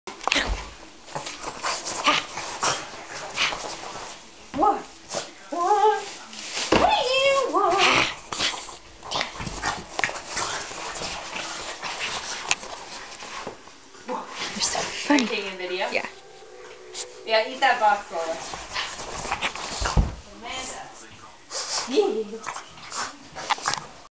Lola worbles
Lola the French Bulldog "worbles" - play attacks - on the bed.  She's such a clown!